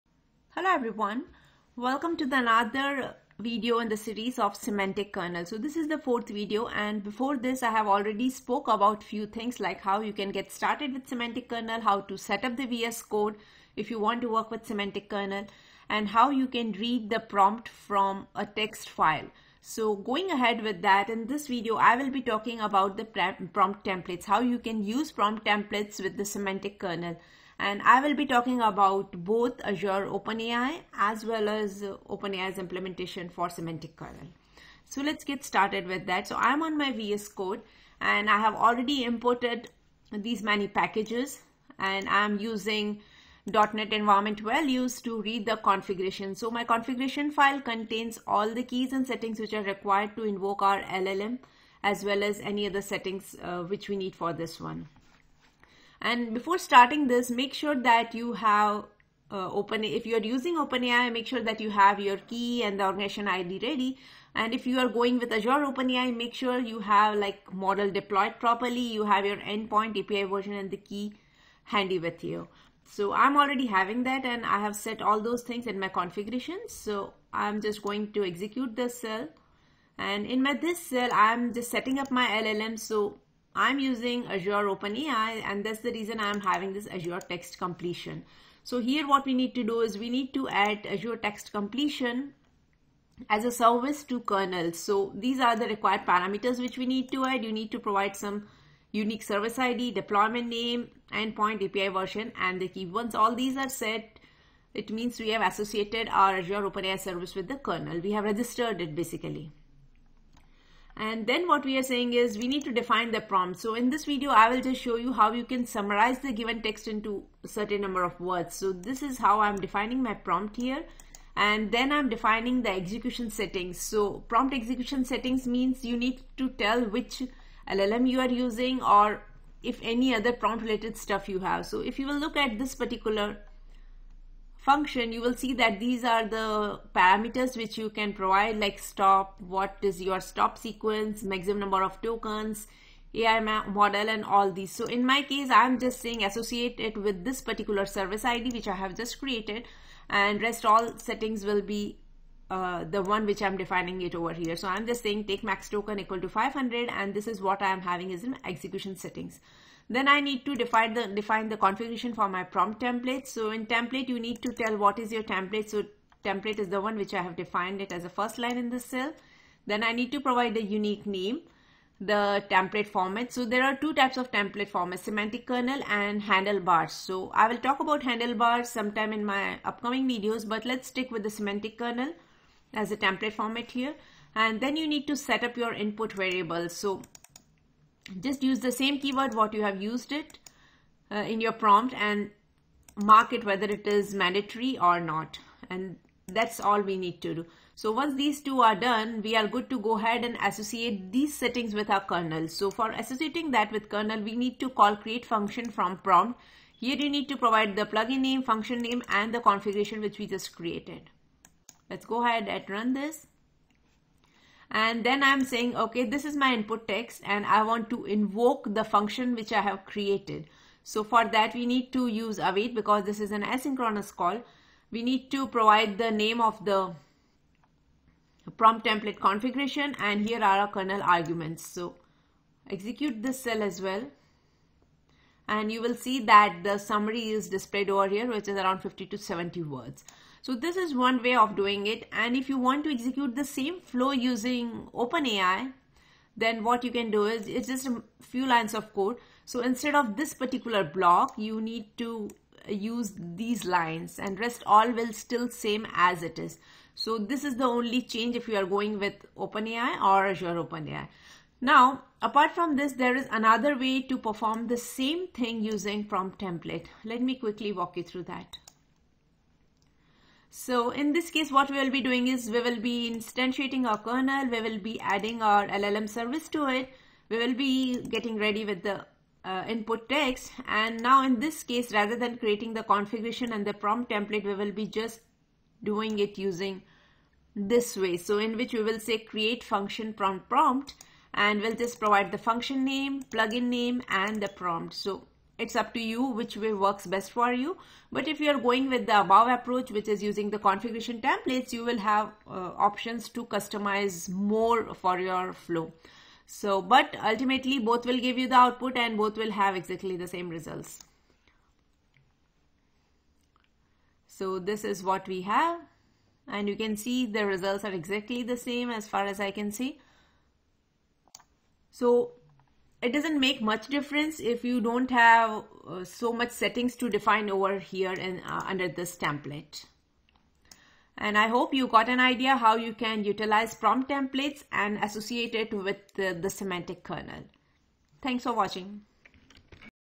04. Using Prompt Templates With Semantic Kernel - OpenAI and Azure OpenAI
Do you have a requirement of writing complex prompts for your LLM? Then check out this video, which talks about how one can use prompt templates with Semantic Kernel.

⭐Python for beginners
⭐Python Pandas
⭐Python tips and tricks
⭐Jupyter tips & tricks
⭐Microsoft Azure
⭐Visual Studio Code a.k.a. VS Code